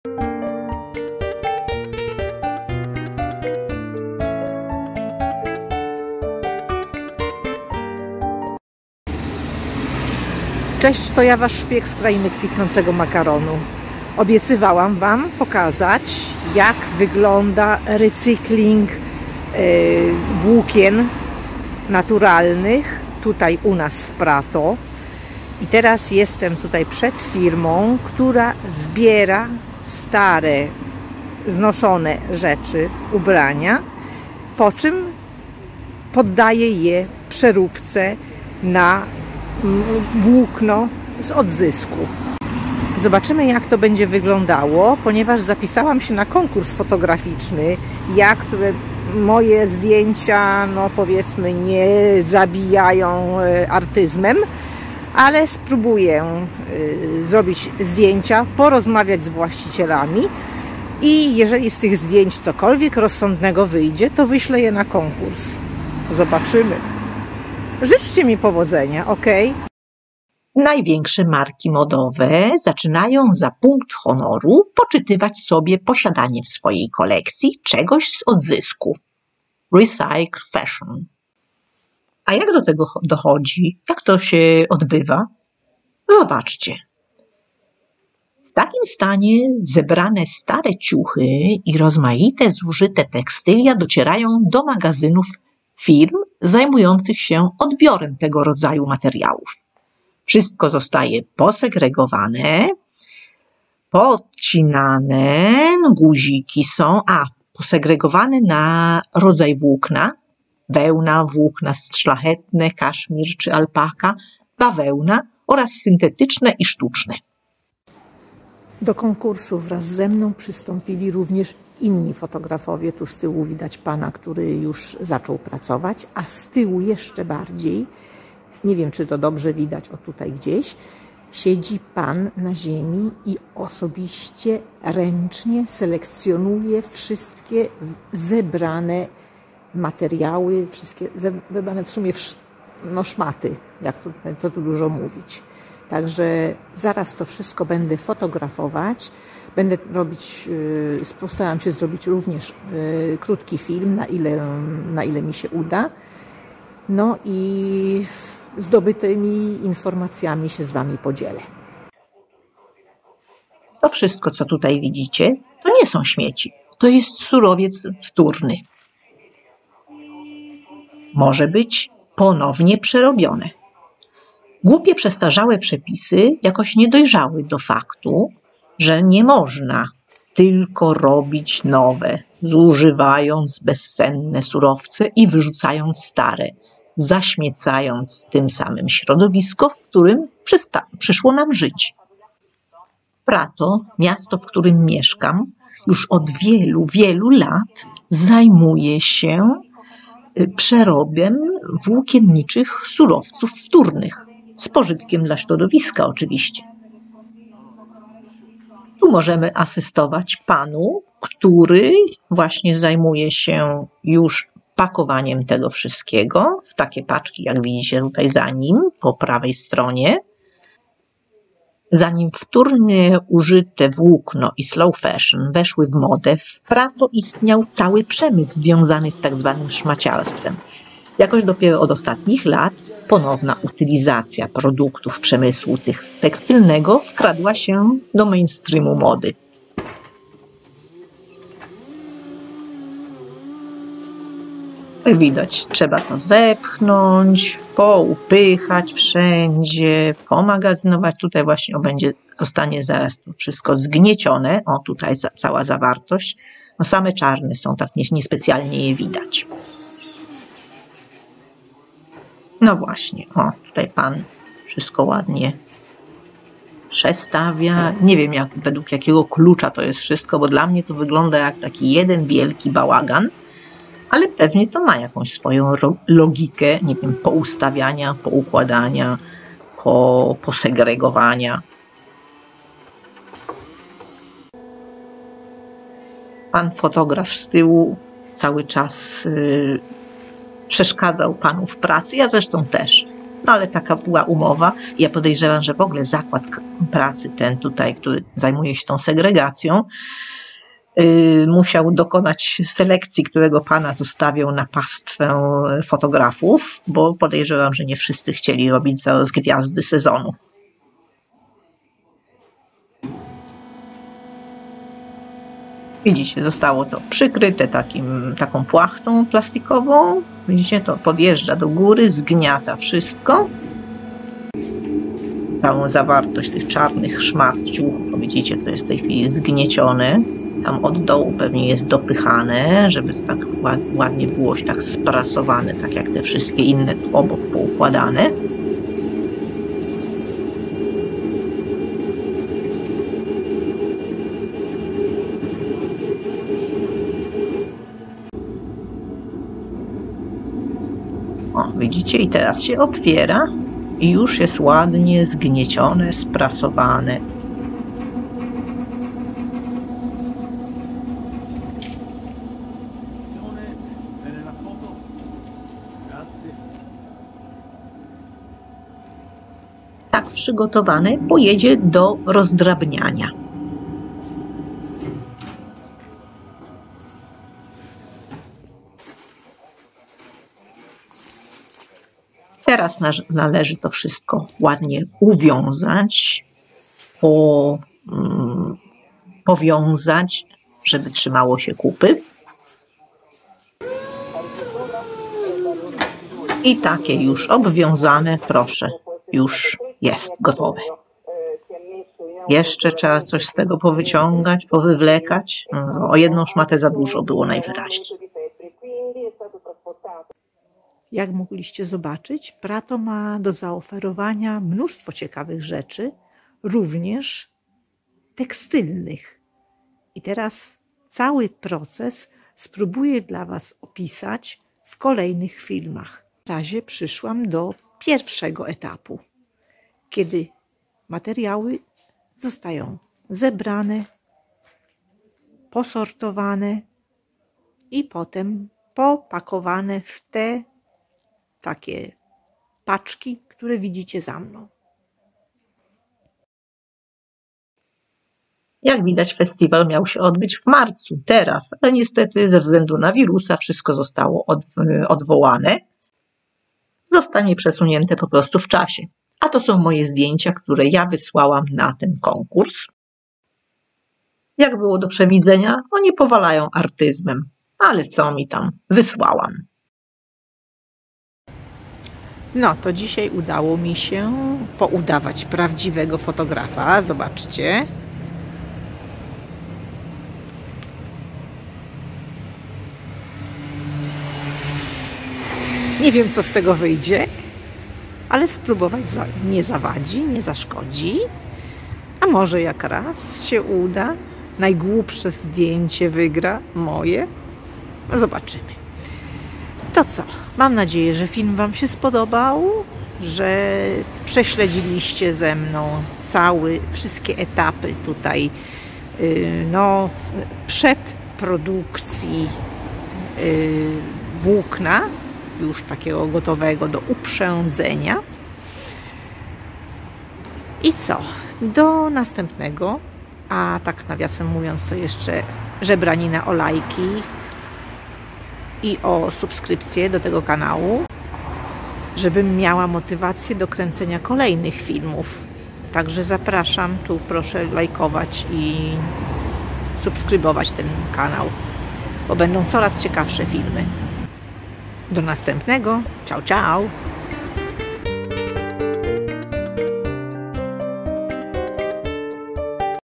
Przemysł recyklingu włókienniczego w Prato
Film zrobiony jeszcze przed atakiem koronawirusa, kiedy można było wychodzić. Przedstawiam w nim pierwszy etap pozyskiwania włókna do ponownego wykorzystania. Kiedy będzie możliwe nakręcenie kolejnych etapów, nie wiem.